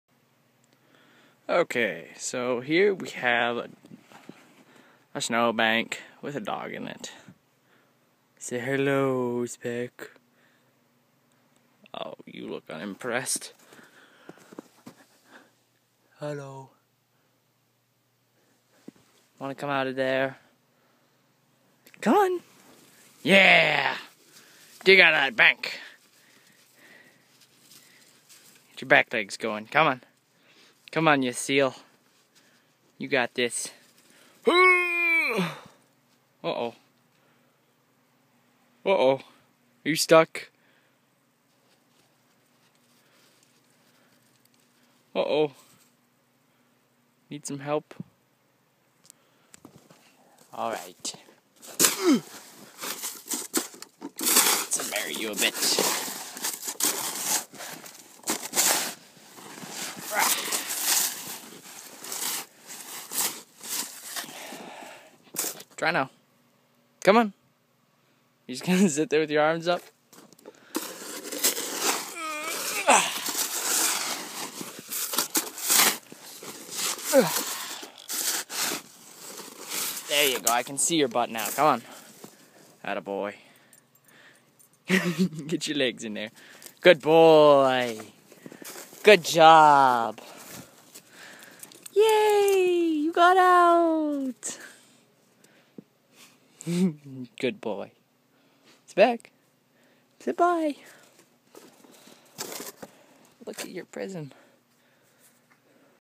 Dog in a snow bank
Yep I buried my dog in a snow bank